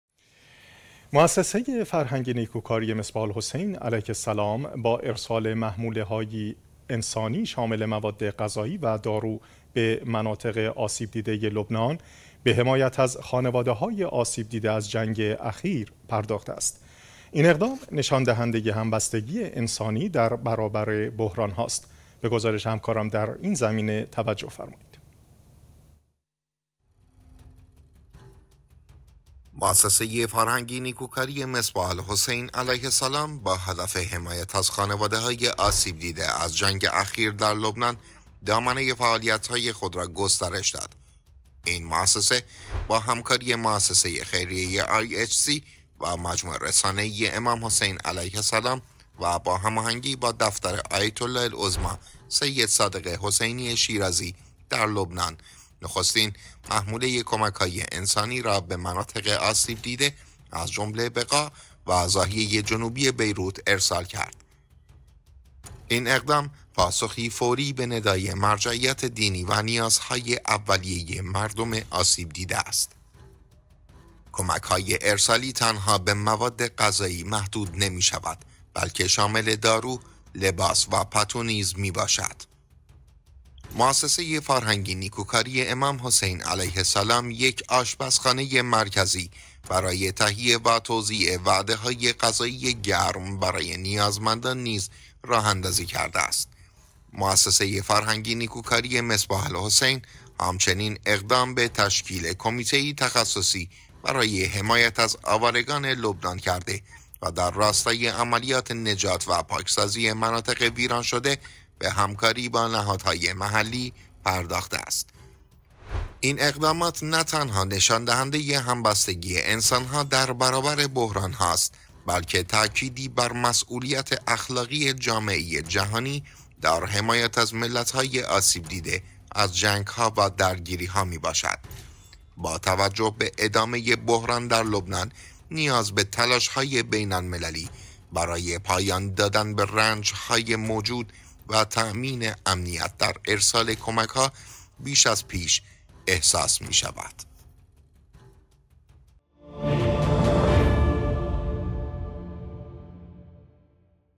کمک‌های فوری مؤسسه مصباح الحسین به آسیب‌دیدگان لبنان
مؤسسه فرهنگی نیکوکاری مصباح الحسین علیه‌السلام با ارسال محموله‌های حیاتی از جمله مواد غذایی، دارو و پتو به مناطق جنگ‌زده لبنان، گام مؤثری در کمک به خانواده‌های آسیب‌دیده از جنگ اخیر برداشته است. این مؤسسه با همکاری نهادهای محلی و حمایت مرجعیت دینی، تلاش کرده است تا از نیازهای اولیه مردم مناطق آسیب‌دیده مانند بقاع و ضاحیه جنوبی بیروت پشتیبانی کند. این اقدامات نمونه‌ای از همبستگی انسانی در برابر بحران‌ها و تعهد به مسئولیت‌های اخلاقی جامعه جهانی است.

اما پشت پرده این کمک‌ها چه تلاش‌هایی در جریان است؟ چگونه مؤسسه مصباح الحسین موفق به اجرای چنین عملیات گسترده‌ای شده است؟ آیا این کمک‌ها توانسته‌اند تغییرات ملموسی در زندگی آسیب‌دیدگان به وجود آورند؟ برای یافتن پاسخ این سوالات و آگاهی از جزئیات بیشتر، حتماً ویدیو و گزارش کامل ما را مشاهده کنید.

ویدیوهای مرتبط

ویدیوهای مرتبط